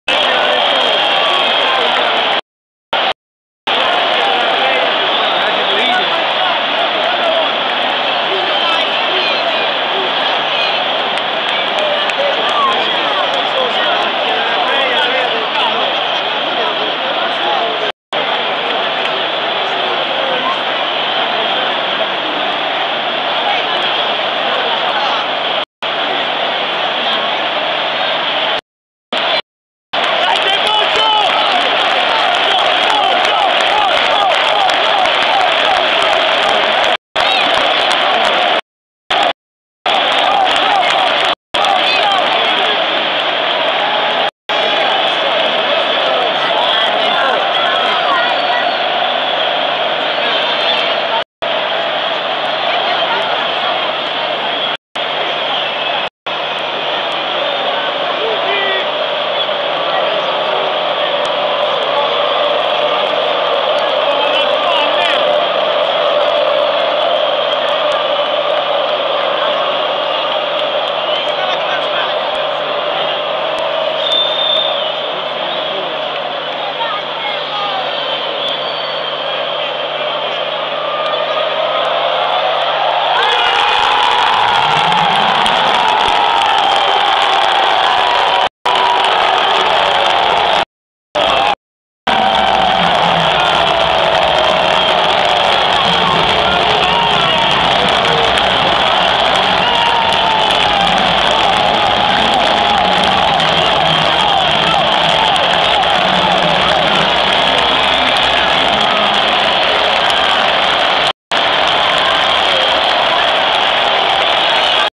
NAPOLI-MILAN 3-1 LIVE TRIBUNA POSILLIPO RIGORE DOMIZZI
DELIRIO AL SAN PAOLO RADDOPPIO DI DOMIZZI SU RIGORE